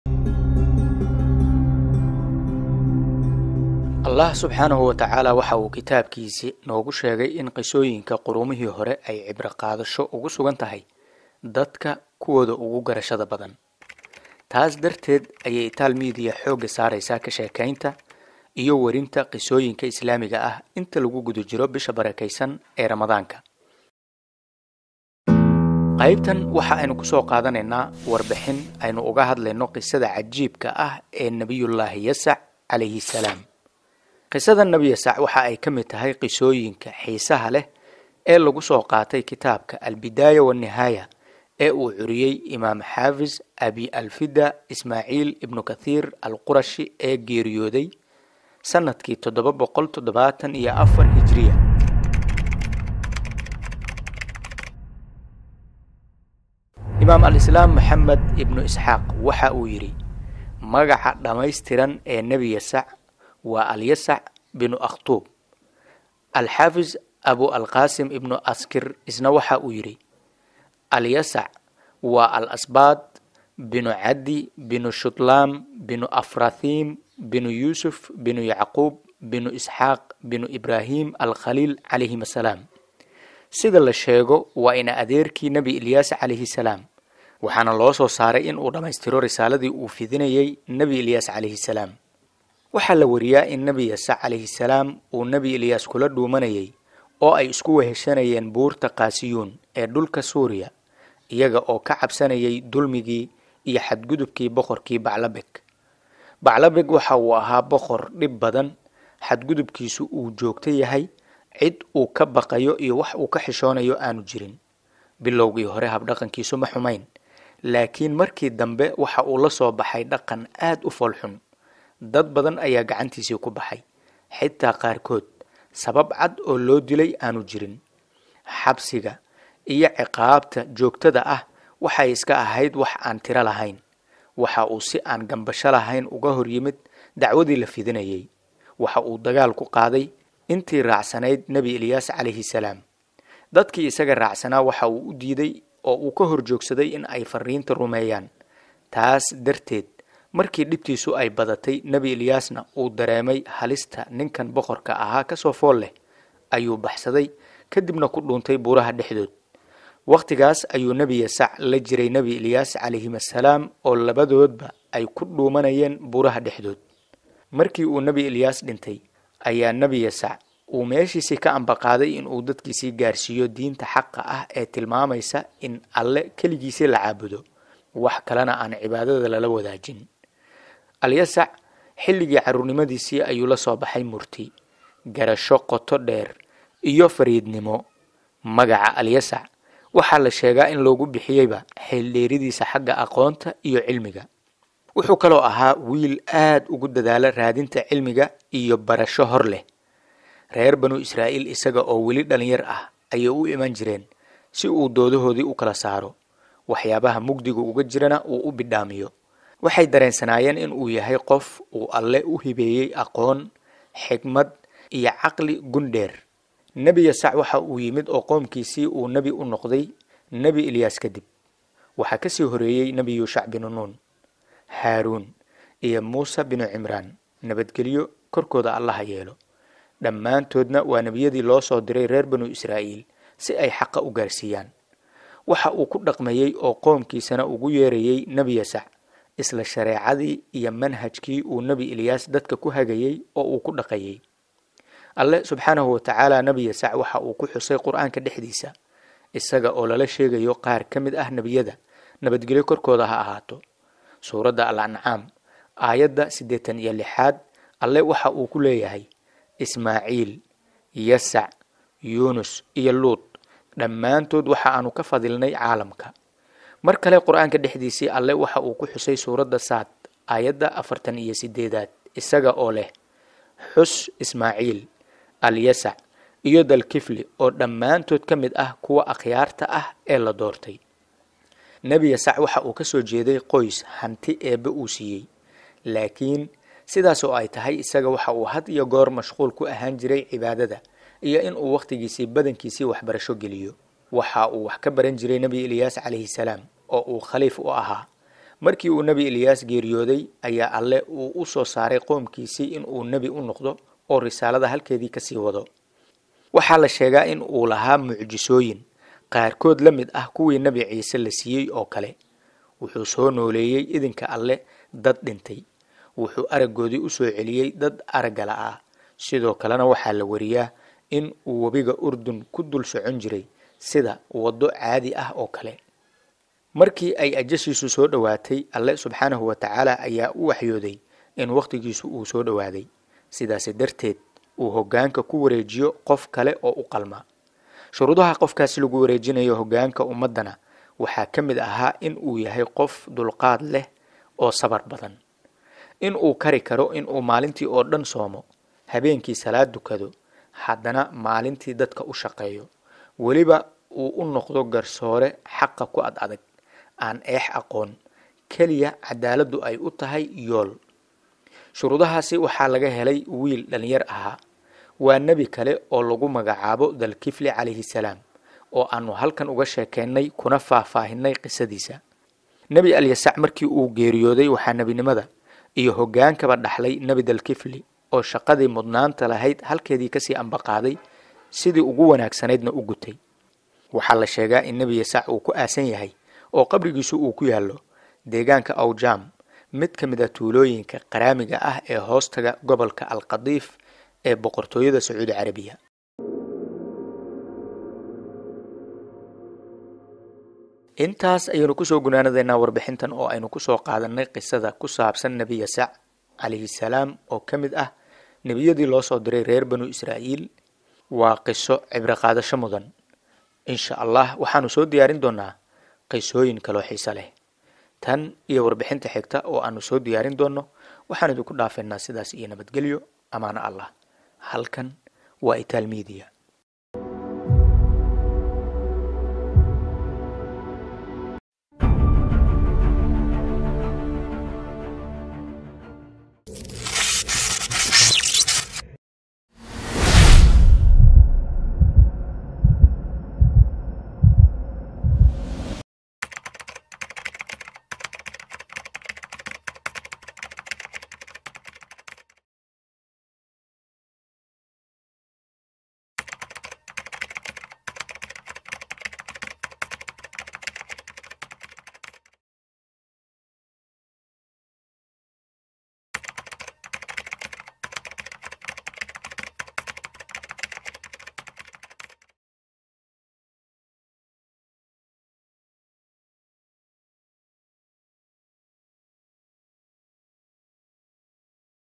Qisada Nabi Yasac Cs | Nabigii dhaxlay Nabi Ilyaas Cs isna uu dhaxlay Nabi Dal-kifli Cs
Qisada Nabi Yasac Cs Nabigii dhaxlay Nabi Ilyaas cs isna uu dhaxlay Nabi Dal-kifli